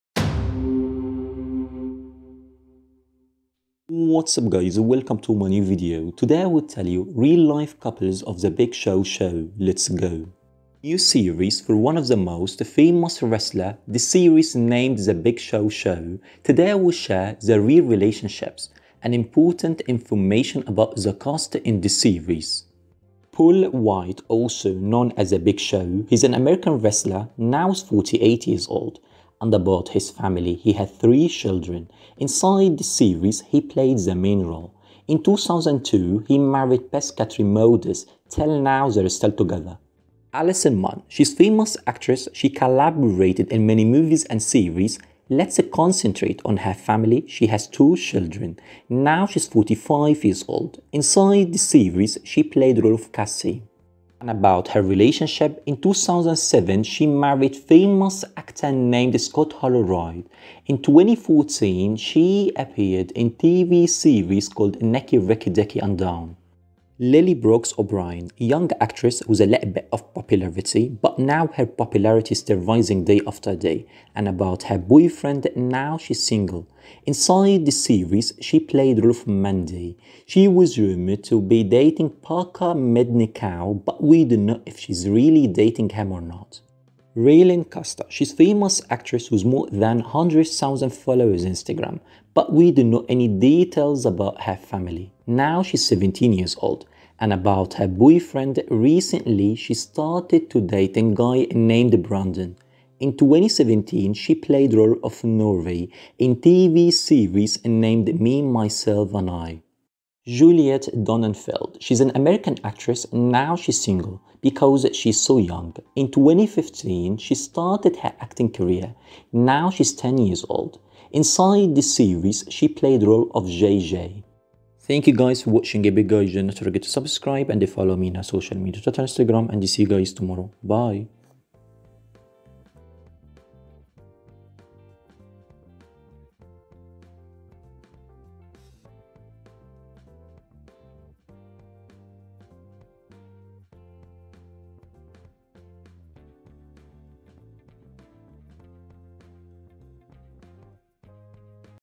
Real Life Couples of The Big Show Show
The Big Show Show
Today I'll tell you about The Big Show Show real age and life partners and more information about the cast inside this series.
The full cast:
Paul Wight
Allison Munn
Reylynn Caster
Lily Brooks O'Briant
Juliet Donenfeld